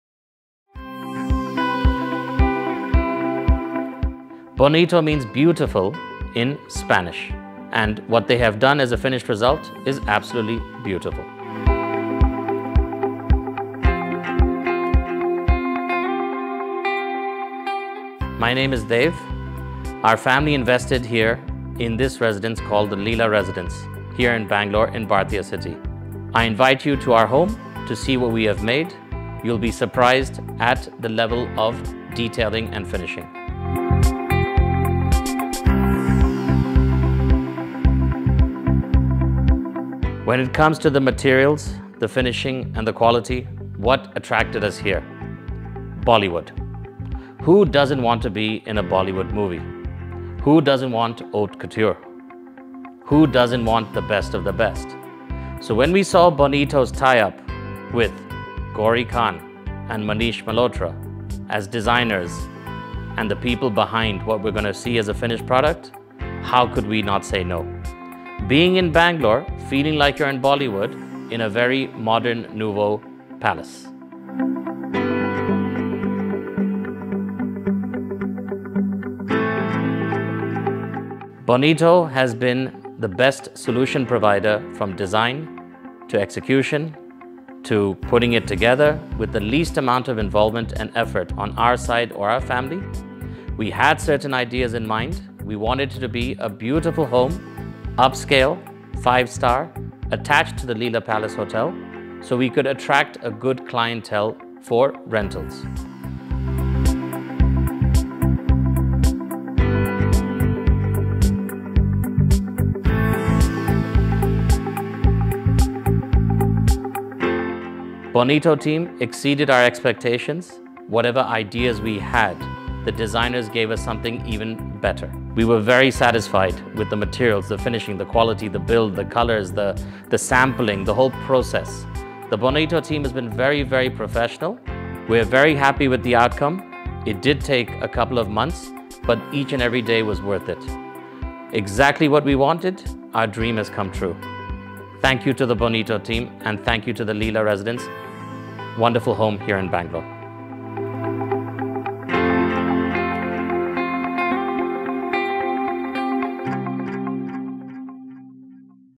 Puya Kirpalani's Dream Home | Bangalore | Bonito Designs
Sometimes all we need is a bit of everything!

The family presented us with an exceptional vision—a craving for a design that harmonizes with an eclectic mix. Departing from the norm of adhering to a singular theme, this unconventional approach not only yielded remarkable results for them but also provided an exhilarating challenge for our team.

The result is in the visual. See for yourself, how the design elements from various design elements came together a design that seamlessly intertwines playfulness and elegance.

It indeed is one of the rare scenarios where more than one design theme makes home interiors a bit more charming.

Click here to schedule your interior design consultation today!

Video Producer - Mahesh Bhambere

READ what our experts have to say!